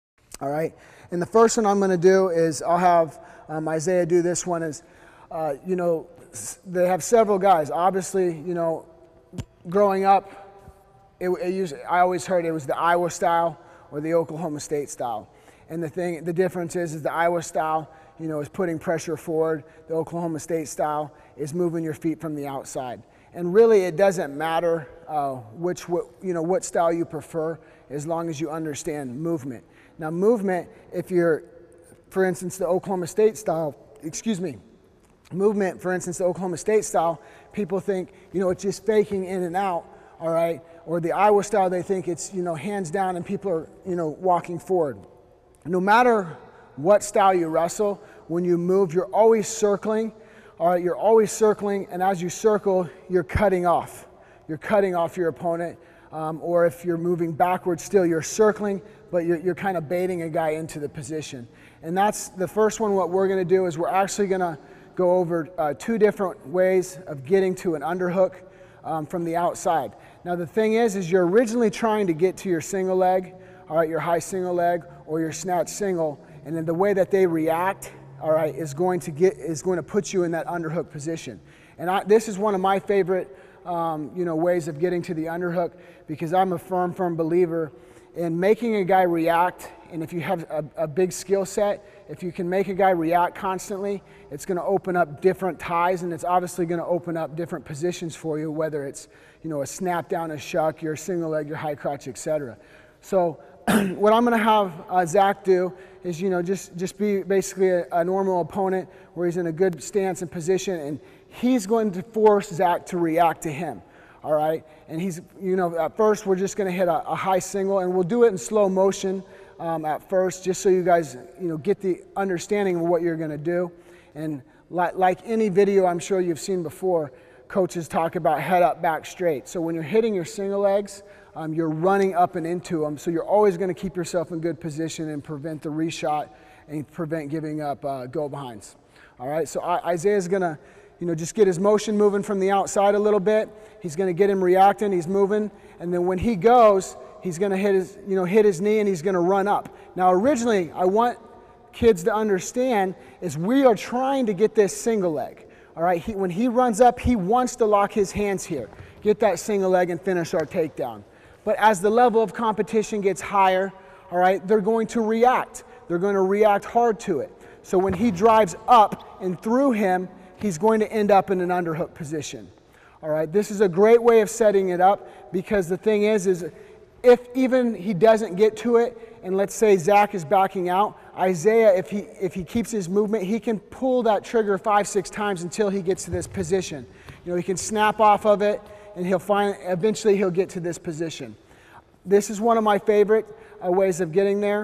Get an Underhook from a Single Leg Shot! - Wrestling 2016 #4
Mark Perry, University of Illinois associate head coach and former two time National Champion wrestler, explains how to use a single leg shot to get an underhook. As opponents become more advanced, you’ll have to find creative ways like this one to get to an underhook position.